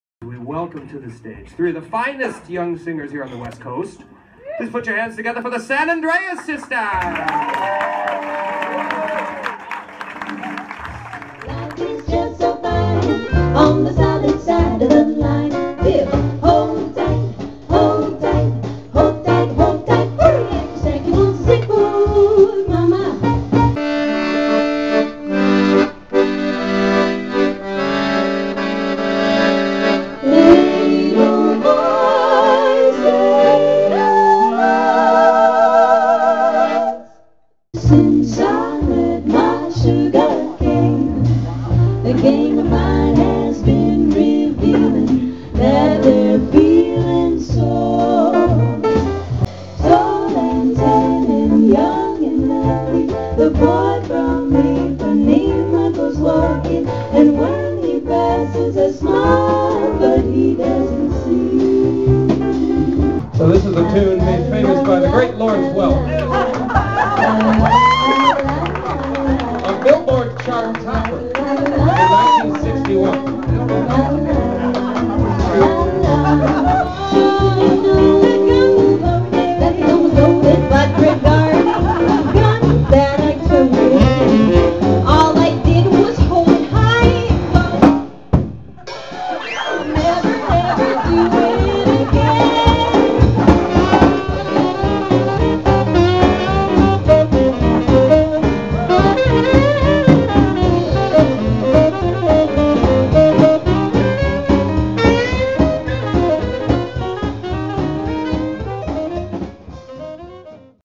The San Andreas Sisters
Clips from The San Andreas Sisters debut show at the E Spot Lounge in Studio City California.
This lovely trio of ladies perform classic tunes from the 1930's-60's.